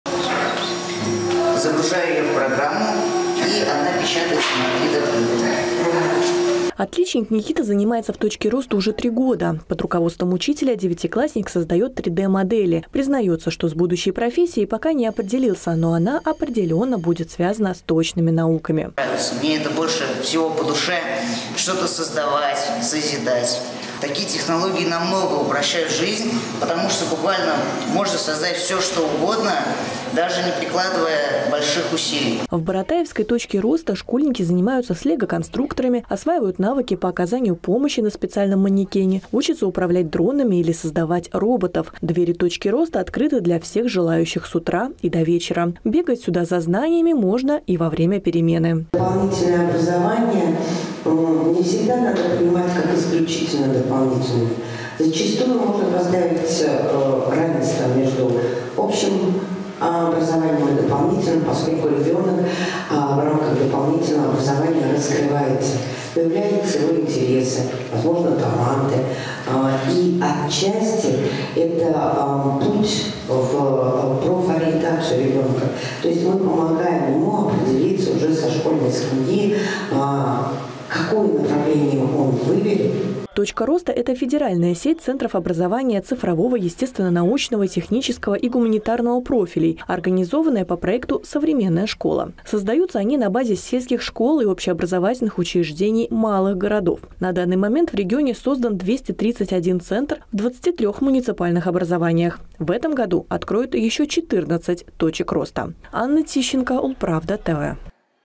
Делают роботов и 3D – модели. В школах Ульяновской области откроют 14 новых «Точек роста»
Обучиться 3D-моделированию или компьютерным технологиям, не выходя из школы. Получить разнообразные навыки и знания, которые не входят в учебную программу, могут не только ребята из областного центра, но и ученики отдаленных сел Ульяновской области. Все благодаря работе «Точек роста». Как они помогают школьникам – в нашем материале.